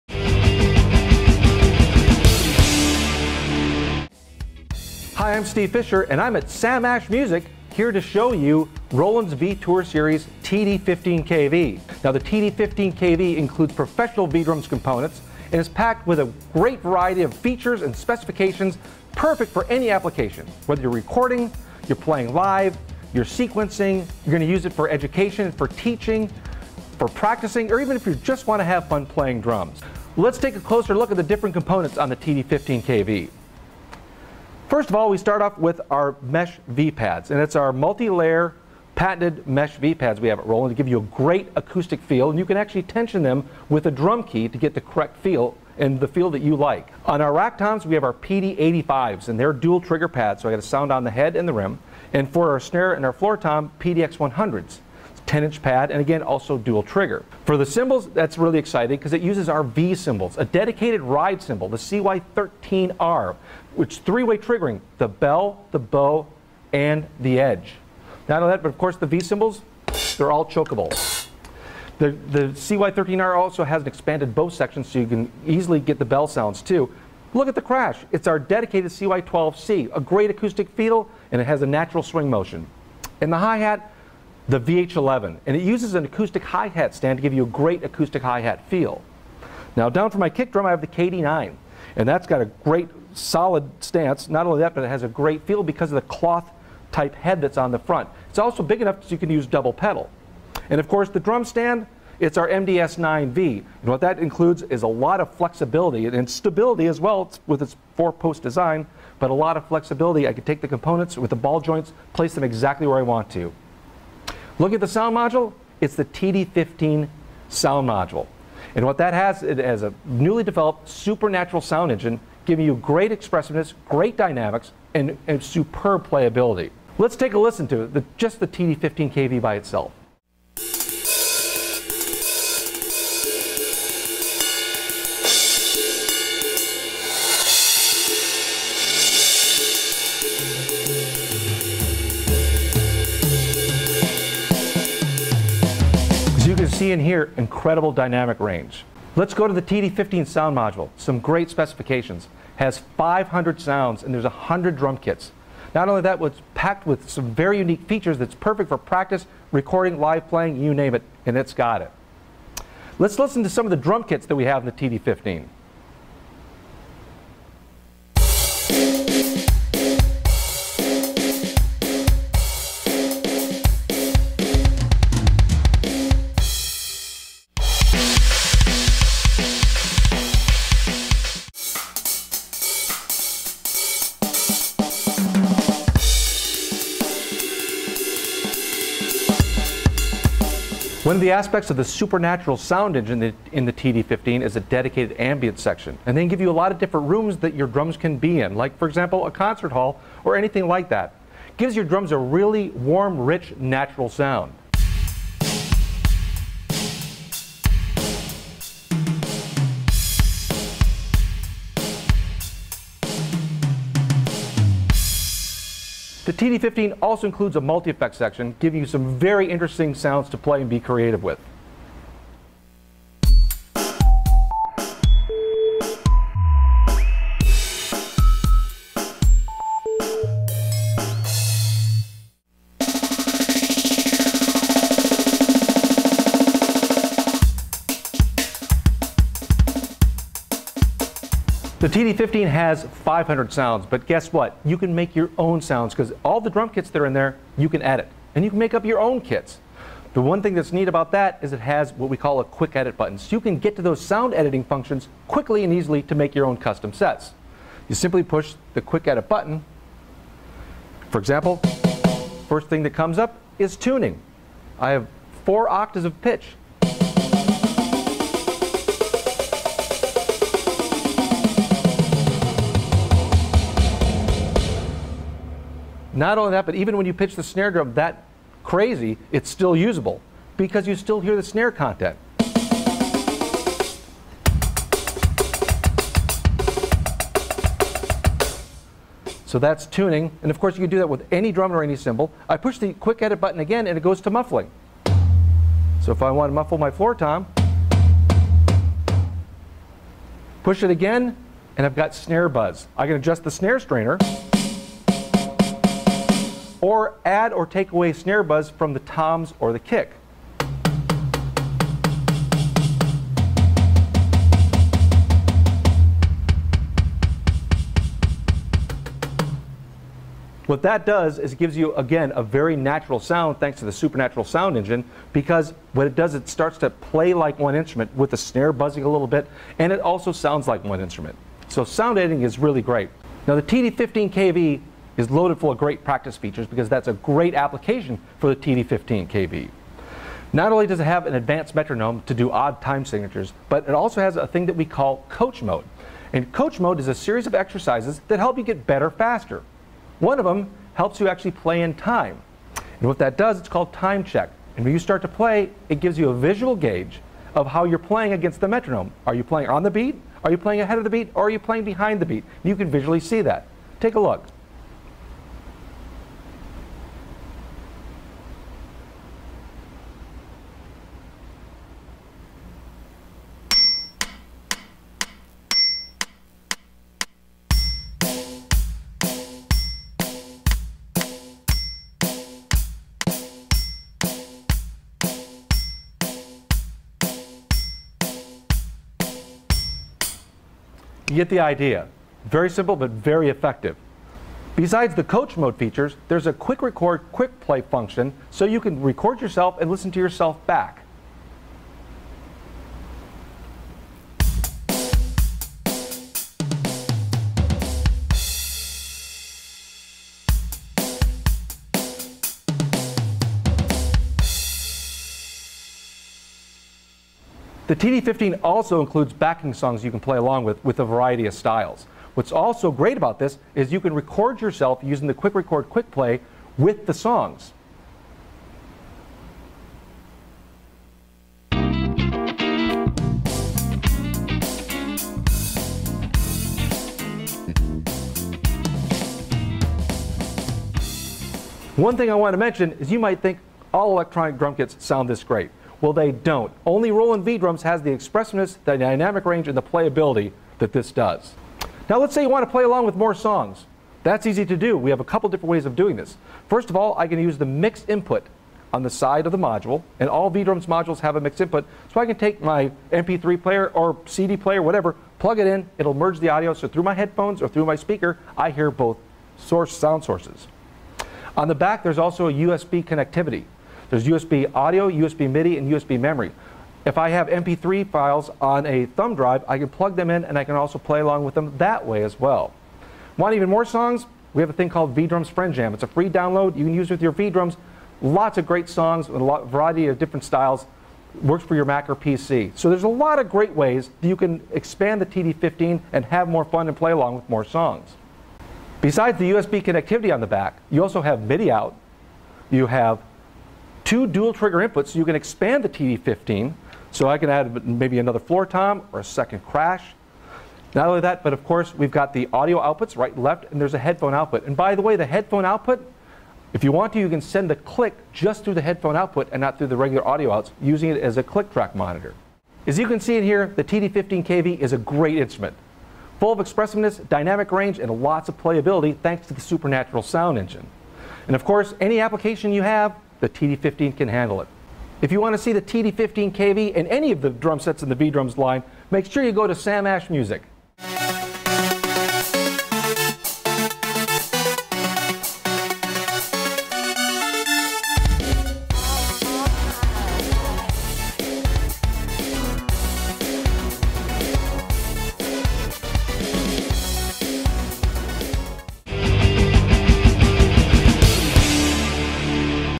Roland TD-15KV V-Tour Series | Everything You Need To Know
The groundbreaking TD-30 V-Drums kit was the first in history to offer SuperNATURAL sounds and Behavior Modeling. Today, the middle range of the V-Drums family inherits this acclaimed technology with the debut of the TD-15KV and TD-15K V-Tour kits. Drummers can now experience flagship SuperNATURAL performance, authenticity, and expression in a more affordable and compact format.

Powered by SuperNATURAL technology, the TD-15 Drum Sound Module provides outstanding playability, superb expressiveness, and pro-level operation. Powerful and affordable, this V-Drums sound engine is ideal for both the live stage and recording studio.

As the snare and floor tom, the TD-15KV equipped with the new PDX-100, a lightweight 10-inch mesh head V-Pad with an advanced dual-trigger sensor, a metal rim hoop, a tensionable head, and a compact casing. For perfect positioning, the dual-mounting capability increases your setup versatility.

The PD-85BK 8-inch dual-zone V-Pad for toms features Roland's acclaimed dual-triggering technology, resulting in more consistent and accurate sensing between the head and rim. Built tank-tough and ready to take a pounding, the mesh head V-Pad sports a cool black finish with chrome bracket. Used in conjunction with the TD-15 sound module, the pad lets you play rimshots, and the sound will change naturally depending on the strength of your stroke.